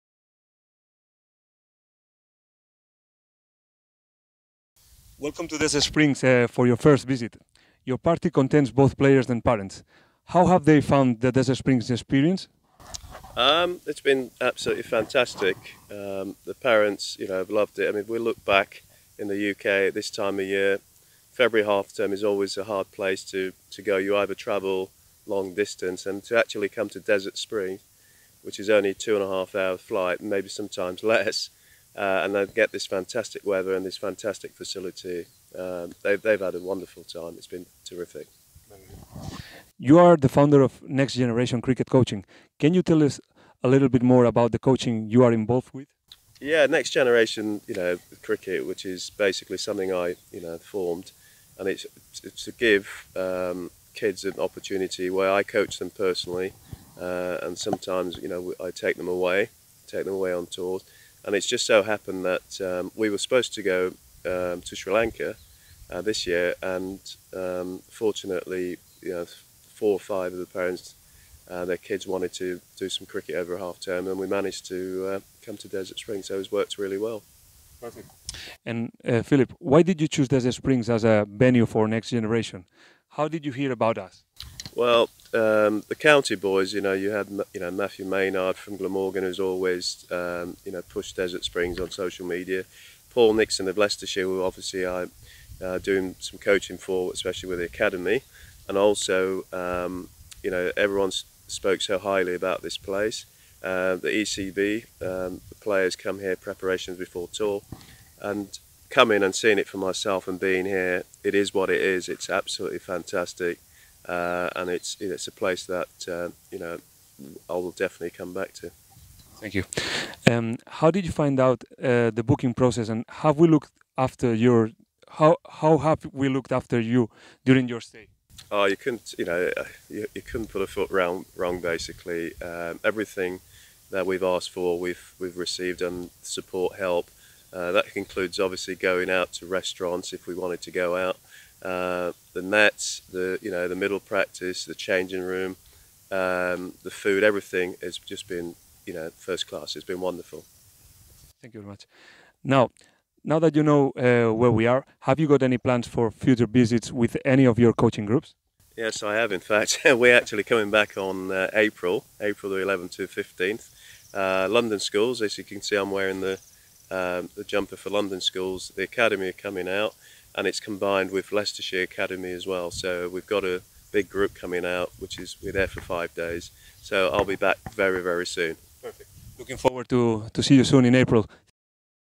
Desert Springs Cricket - Team Talk with Phil Defreitas
Phil Defreitas Next Generation Cricket Development - warm weather training at Desert Springs Cricket Oval and Academy